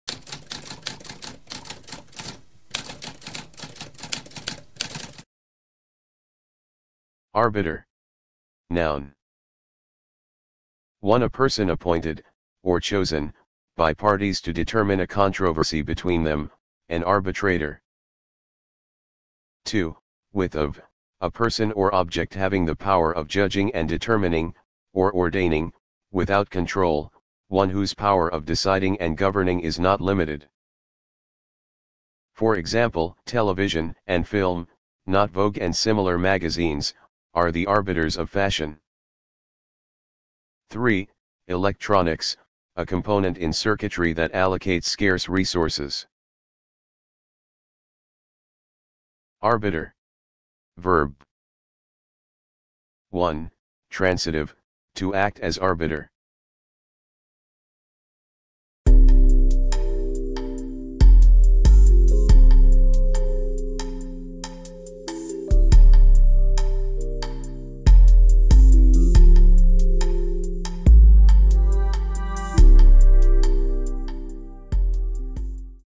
What does arbiter mean?
A spoken definition of arbiter.

Intro Sound:
Typewriter - Tamskp